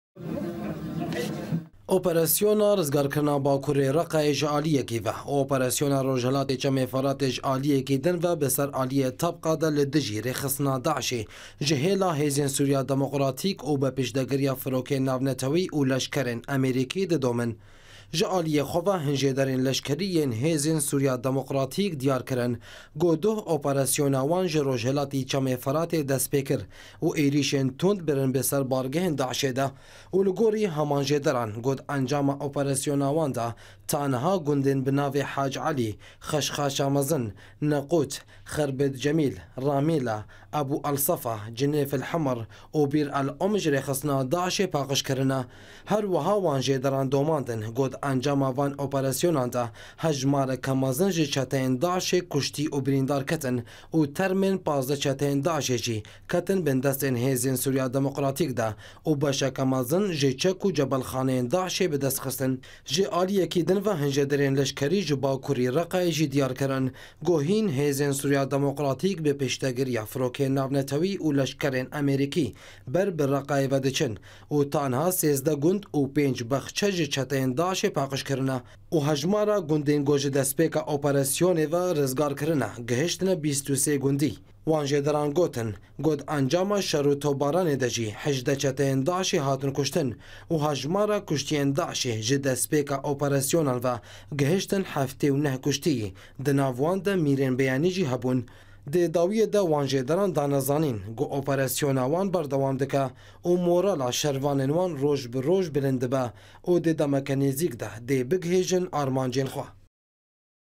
Li bkurê Reqayê HSDê bi piştegiriya firokeyan bi pêş dikeve
Li gundewarê Reqayê, Operisyona hêzên Sûriya demoqratîk bi piştegiriya
firokeyên navnetewî û leşkerên Emerîkî ji aliyê Eyn Îsa û Rojhelatê çemê
Feratê berdewam dike ,,û ji destpêka operisyonan ve û ta îro 23 gund ji
DAIŞê paqij kirin û 79 çeteyên DAIŞê jî kuştin